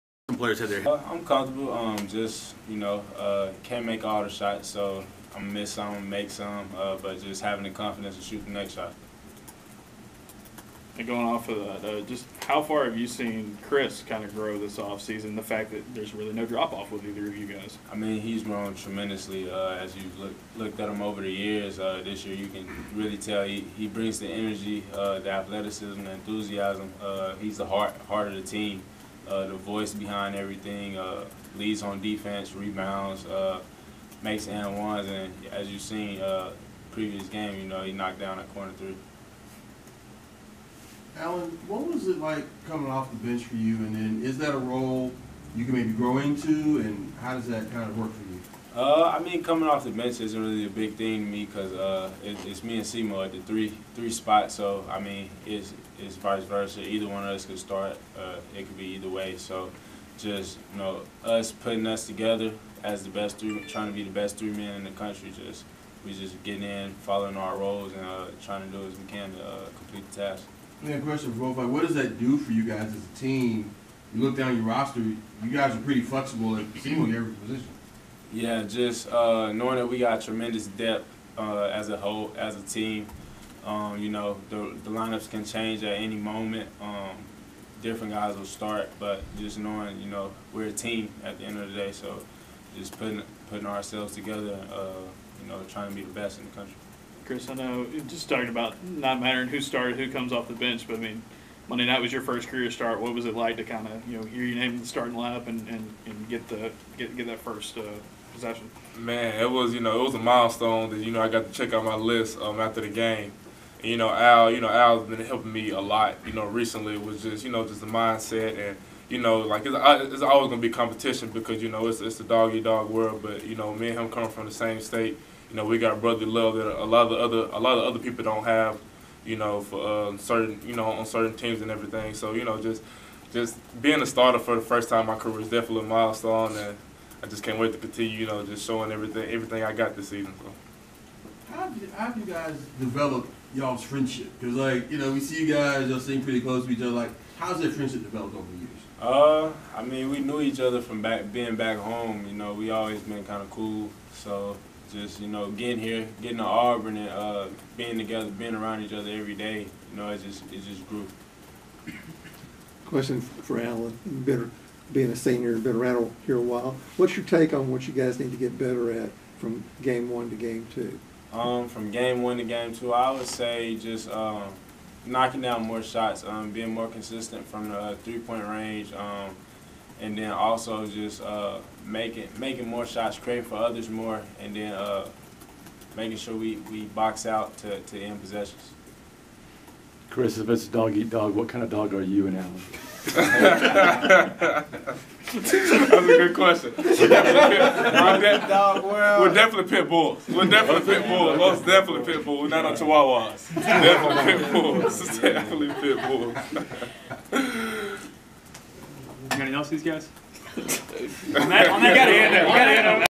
Chris Moore, Allen Flanigan Press Conference 01 3
Junior small forward Chris Moore (left) and senior small forward Allen Flanigan discuss the Auburn basketball season as the Tigers head into game two vs. the South Florida Bulls. The players, who are both from the state of Arkansas, are competing with each other for playing time.